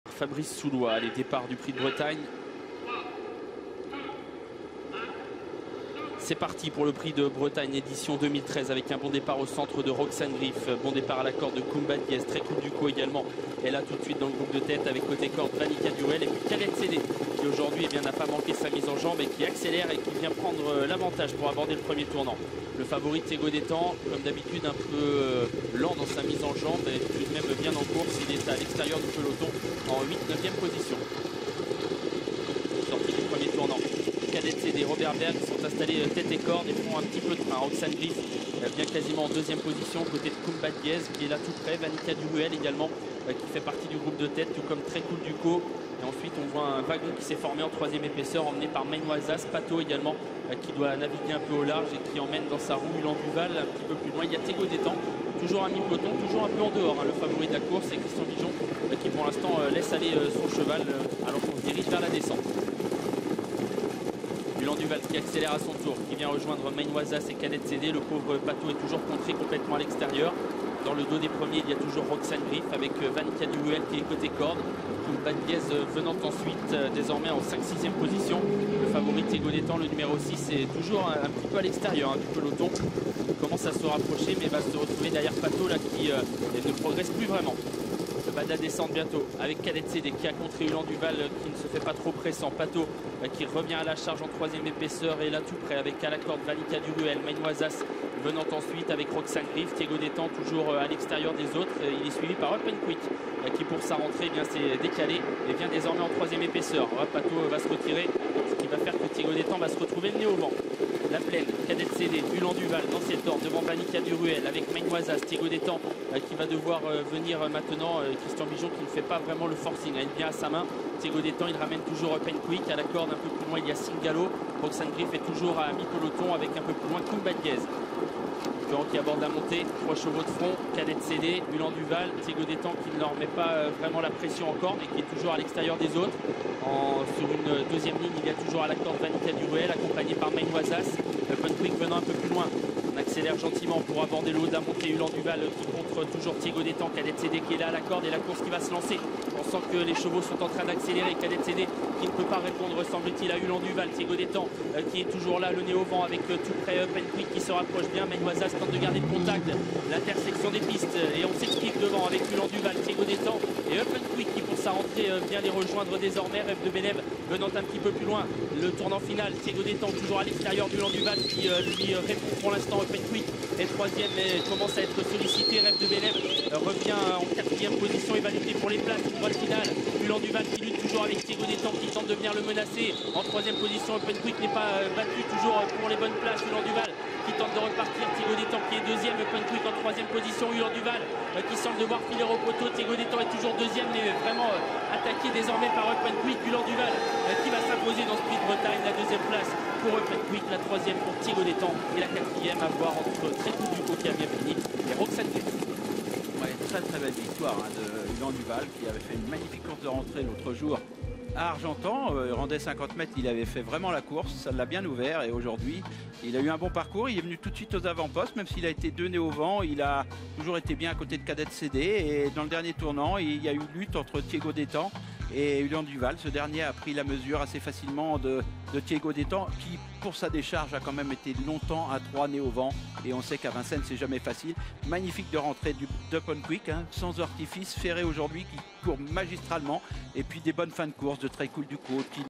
2013 GRAND PRIX DE BRETAGNE - LA COURSE - UHLAN DU VAL
UHLAN DU VAL et C. MEGISSIER s'imposent dans le PRIX DE BRETAGNE à PARIS-VINCENNES (R1C5) - Samedi 16 novembre 2013.
Le Prix de Bretagne devient Prix d'Amérique Races ZEturf Qualif 1